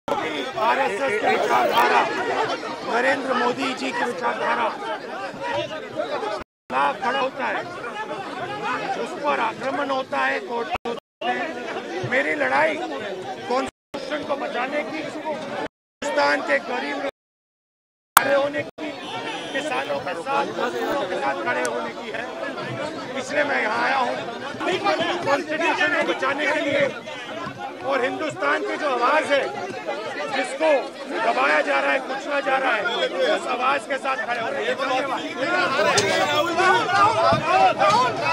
My fight is to save the Constitution: Rahul Gandhi addresses media after the court hearing
Whoever stands against RSS' and Narendra Modi ji's ideology is attacked, court cases are slapped. My fight is to save the Constitution, to stand for the poor & the farmers: Rahul Gandhi addresses media after the court hearing in Patna, Bihar